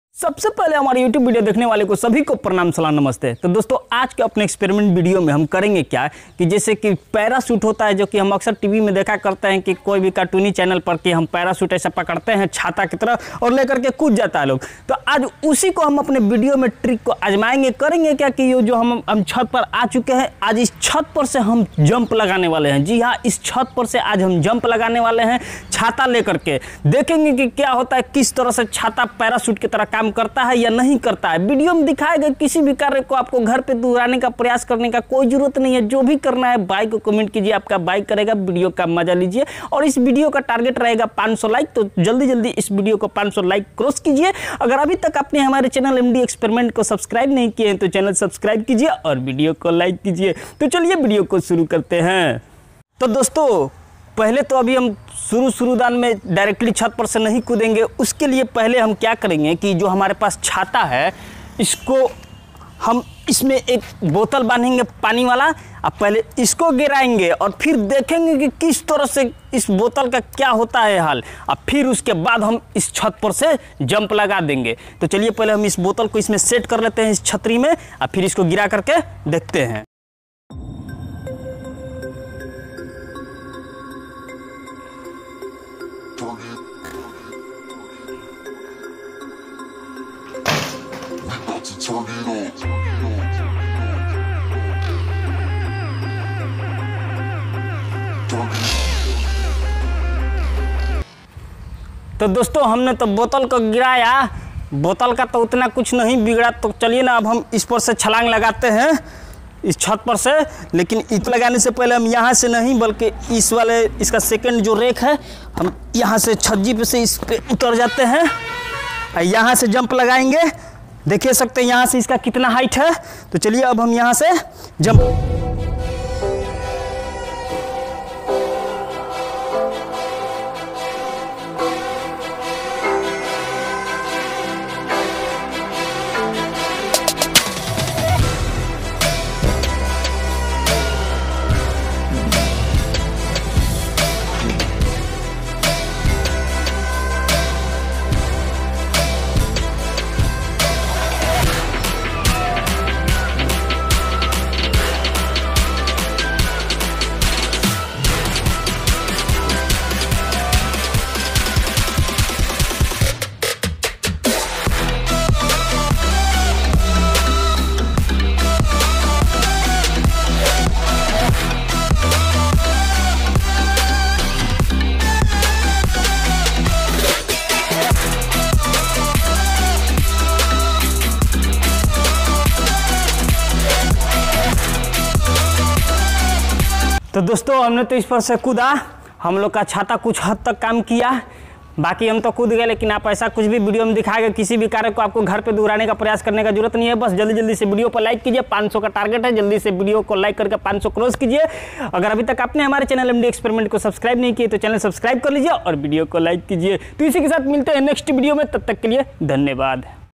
Jumping From Roof with Umbrella (Parachute) -Will it End Me | Md Experiment
इस वीडियो में मैं परीक्षण करता हूं कि पैराशूट के बजाय छतरियों का उपयोग करना संभव है या नहीं।
aor mai chht s jump lga deta hu baki aap khud dekh lo aor mjy lo aor channel pr new aaye hai to subscribe jrur kr lijiyga aor video psnad aay to like thok dijiyga.